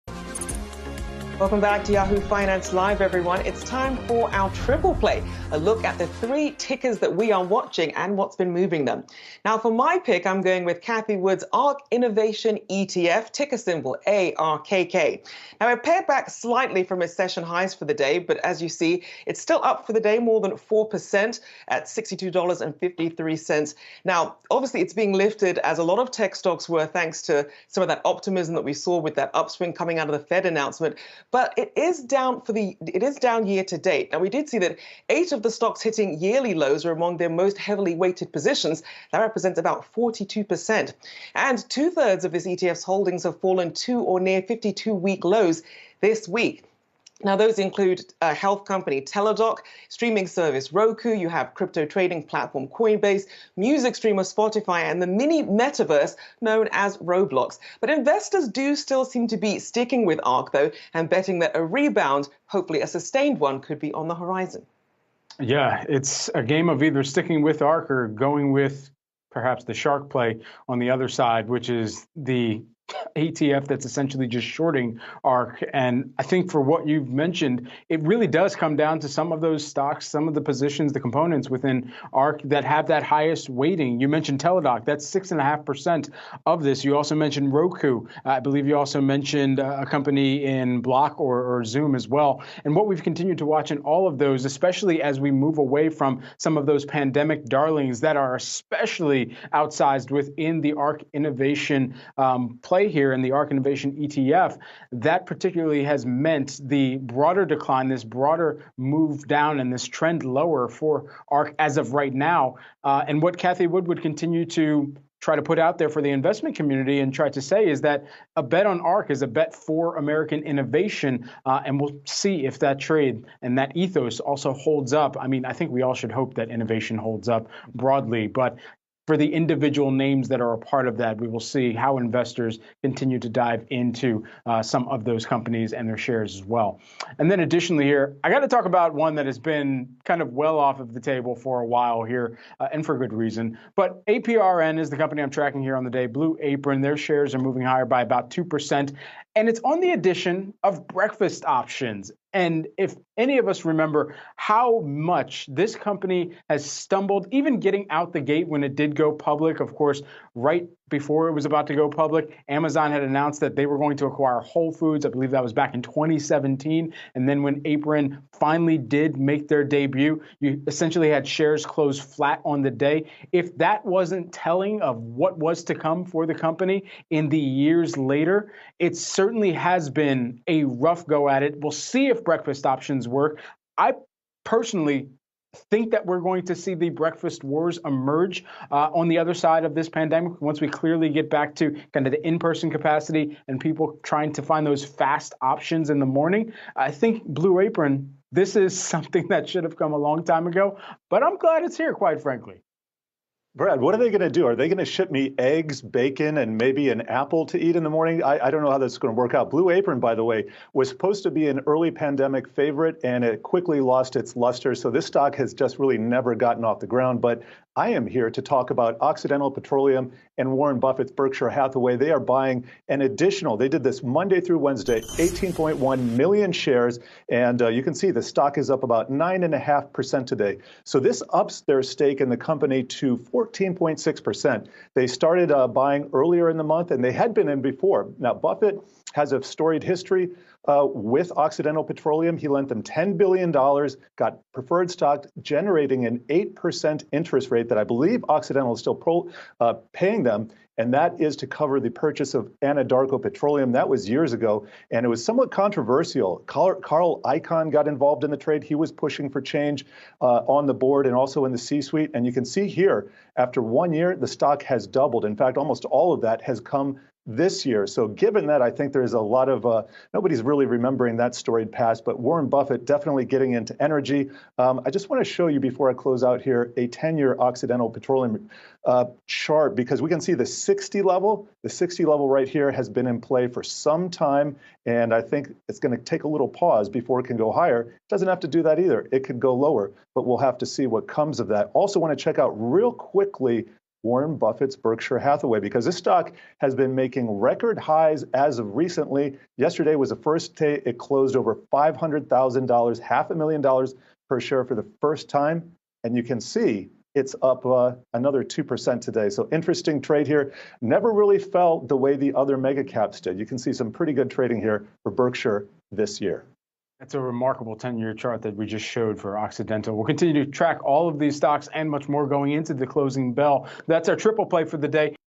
Ark Invest down over 30% in 2022, Blue Apron adds breakfast, Buffett spends $1B more on Occidental
Yahoo Finance Live looks at several of today's trending stock tickers for March 16, 2022.